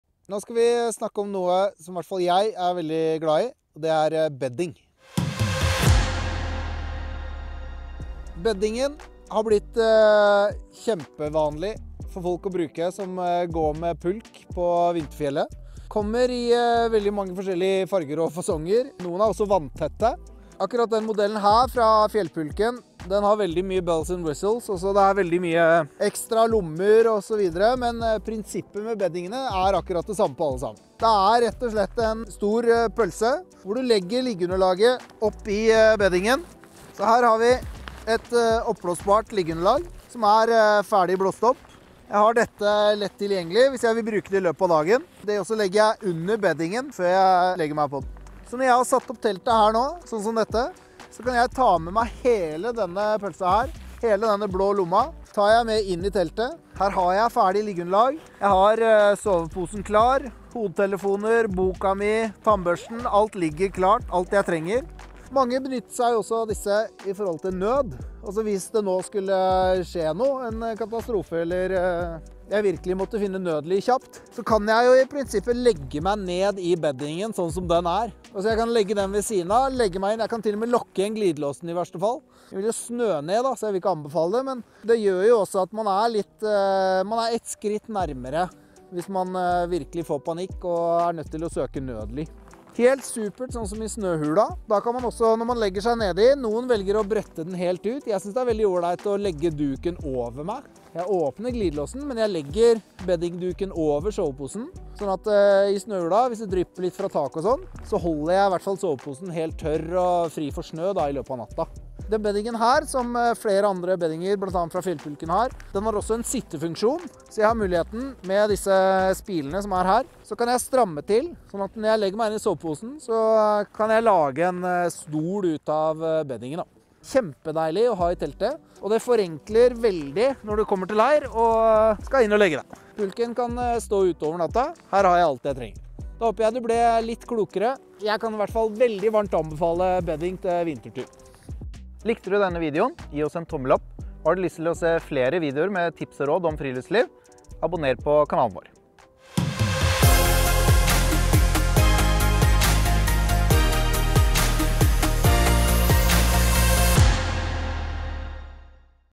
Hvorfor er bedding genialt på vintertur? Oskar forklarer | Fjellsport.no
Bedding er genialt! Det er en rektangulær pose som du fyller med det du trenger inne i teltet. Sovepose, liggeunderlag, bok, liten hodelykt, tannbørste ++. Da ligger alt ferdig oppslått inne i beddingen og du slipper å stappe sovepose og rulle liggeunderlaget på morgenen🎉 Under transport fraktes beddingen som regel på toppen av pulken.

Mange beddinger har også en sittefunksjon (litt som på Exped Chair Kit) som gjør teltlivet veldig behagelig.

God tur! 🏕